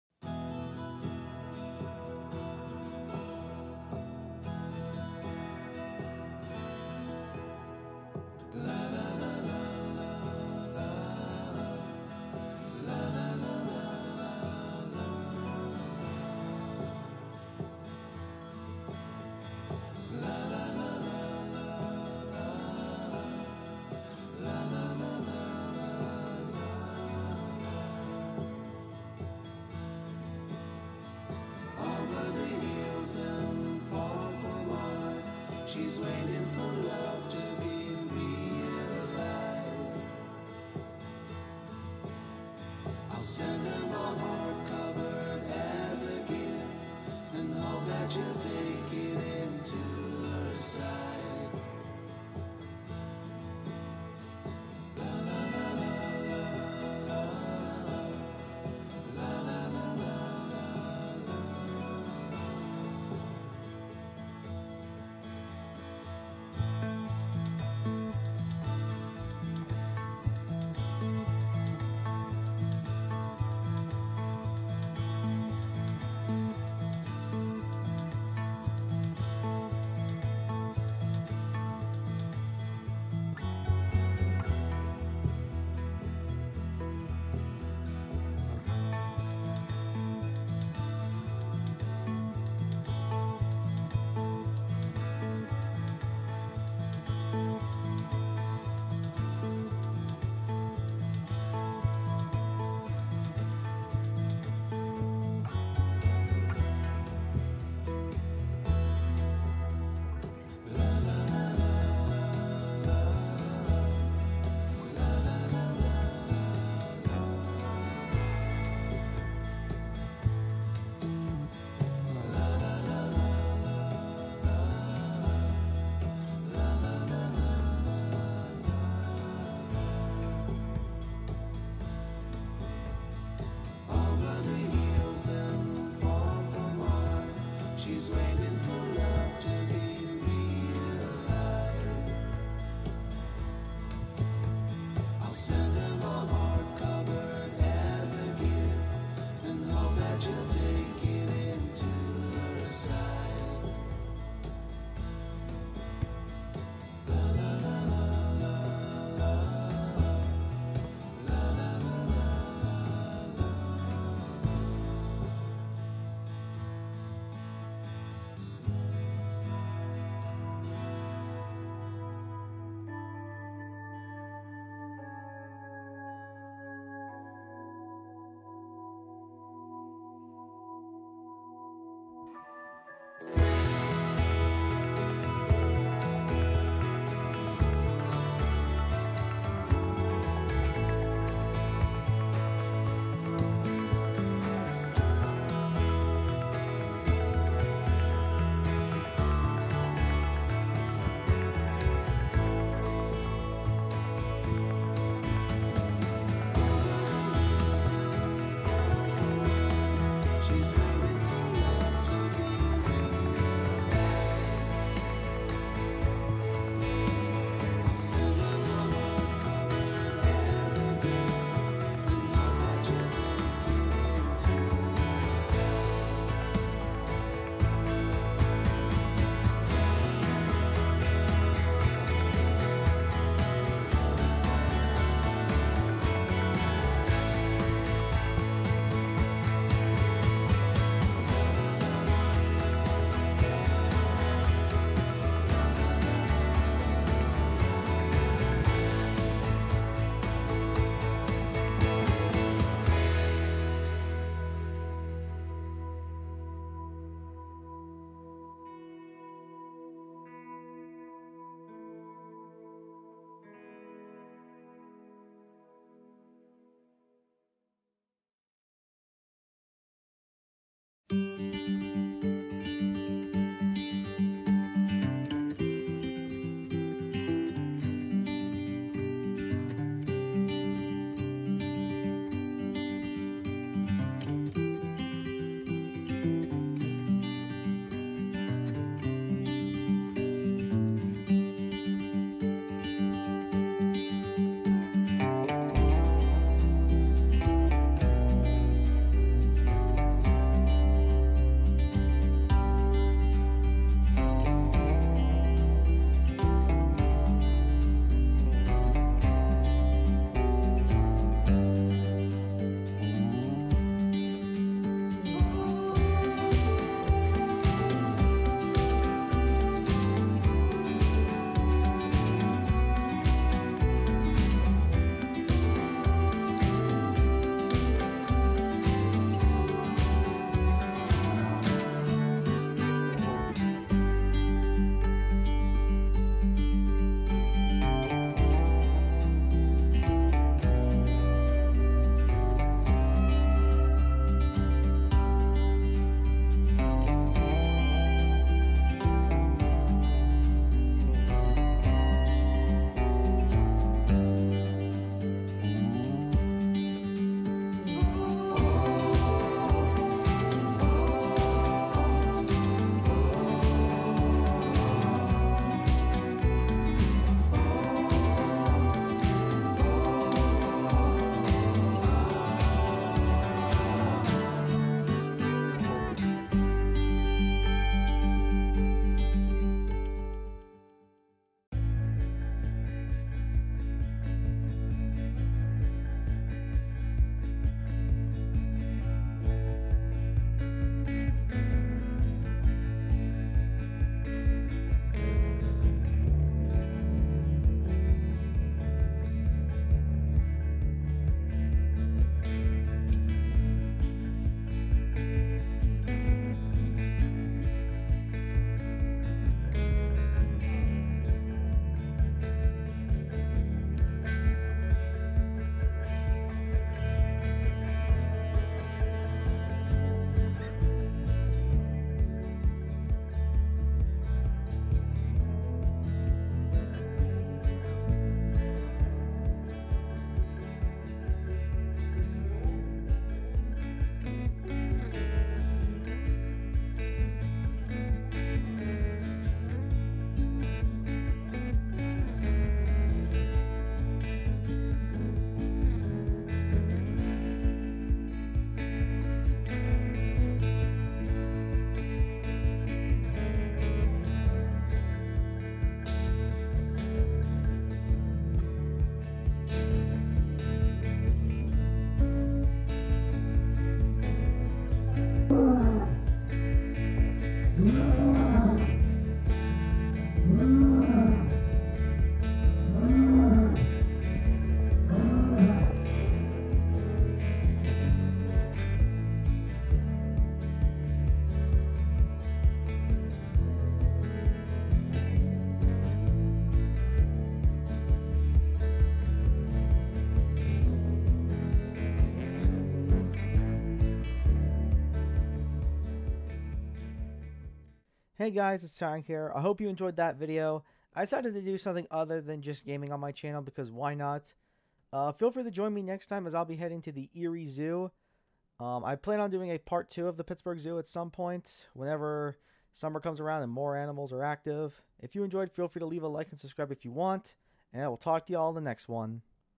A Day Out Episode 1: Pittsburgh Zoo & PPG Aquarium
A new series on the channel I have decided to make. I plan on doing these kinds of videos from time to time. For this first one, me and my mother went to the Pittsburgh Zoo for the first time in 3 1/2 years. Next time, I'll be heading to the Erie Zoo.